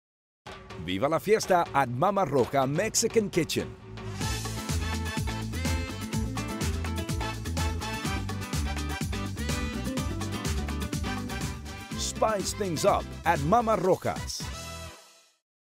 Mama Roja Mexican Kitchen
Dazzle your taste buds at Mama Roja Mexican Kitchen. Restaurant enthusiasts will agree that Mama Roja boasts the best border-style Mexican cuisine Oklahomans have tasted. The border-theme continues in vendor-style tacos, hand-rolled tamales and signature meals in a festive Mexican décor that welcomes guest and provides a resort-style atmosphere. A harbor-side cabana is the perfect spot to enjoy many a sunset on Lake Hefner. Mama Roja would like to invite everyone to join the festivities and "Dress to Chill!"